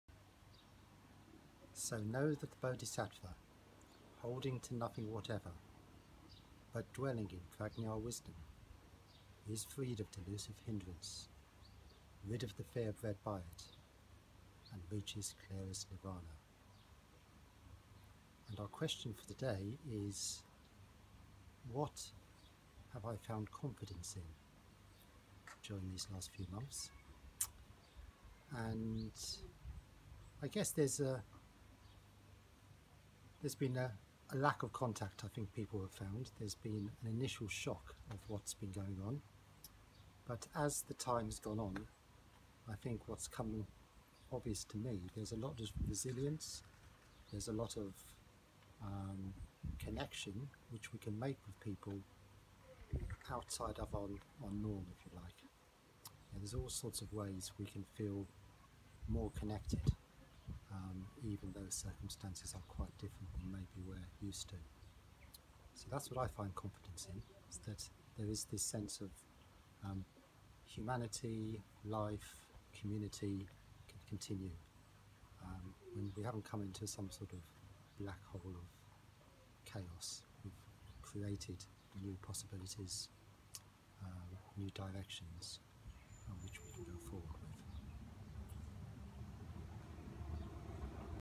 Day 4 - What have you found confidence in?
Today we have Vimalaraja with the question: What have you found confidence in?

And Instagram here: @buddhafieldintheweb